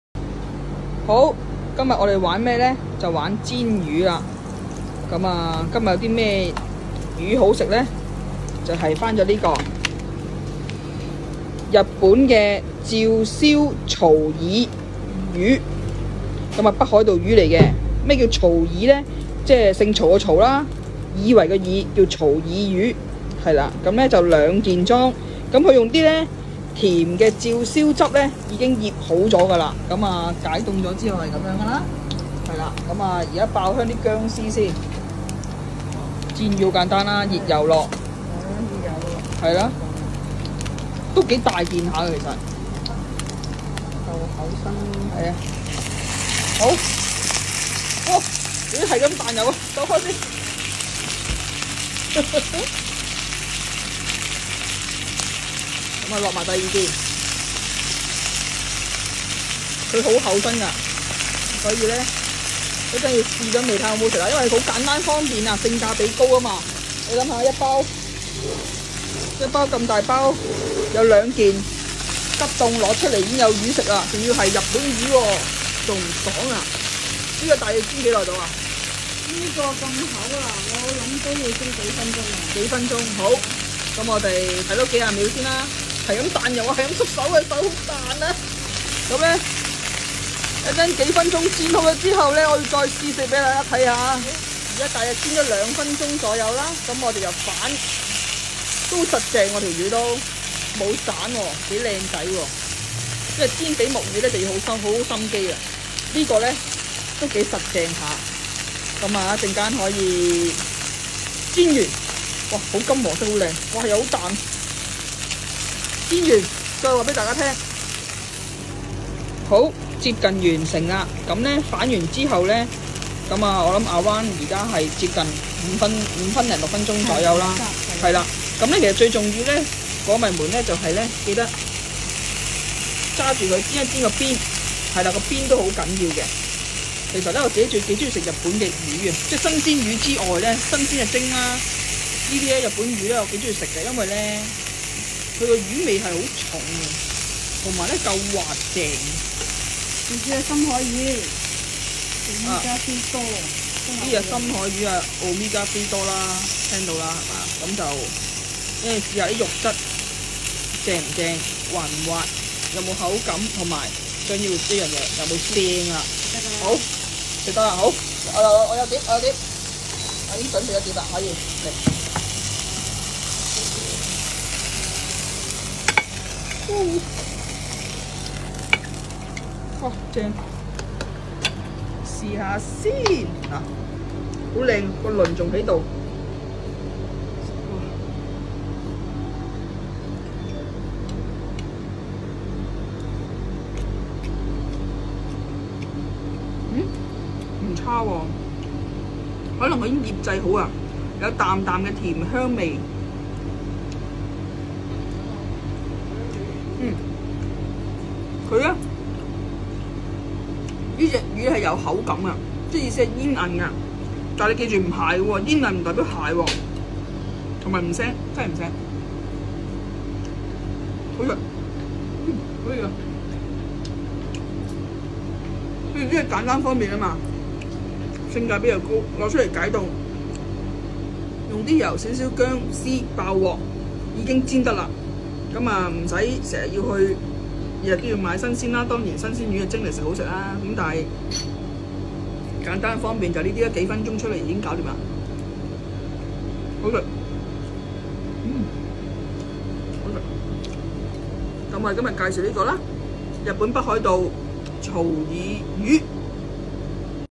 日本北海道照燒曹以切魚
我最鍾意食魚🐟但一定要靚魚😌因為我最怕魚腥味🥺今日介紹另一款日本魚😜立即去片🎬

以甜味照燒汁醃製好，口感滑而爽😋絕對唔[鞋口］😂香口野味😋
適合燒、煎及烤焗